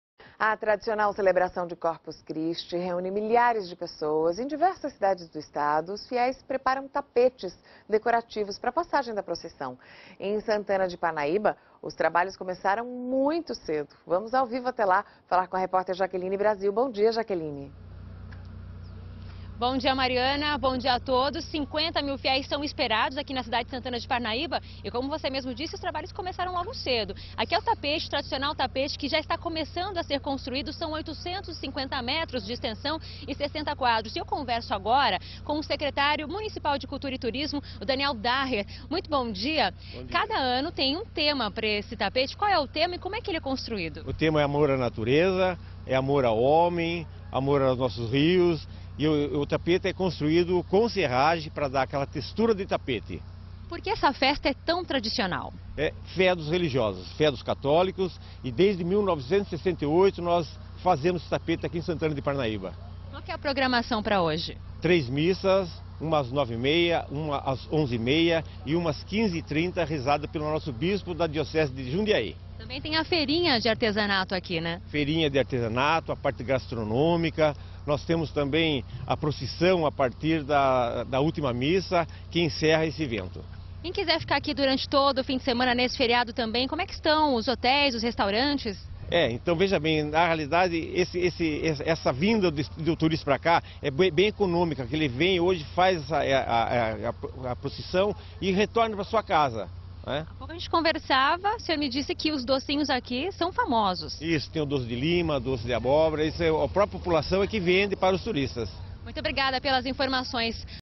Corpus Christis 2011 - Entrevista com o Secretário de Cultura e Turismo Daniel Daher.
Corpus Christis 2011 - Primeira entrevista do dia em Santana de Parnaíba com o Secretário de Cultura e Turismo Daniel Daher (Deco).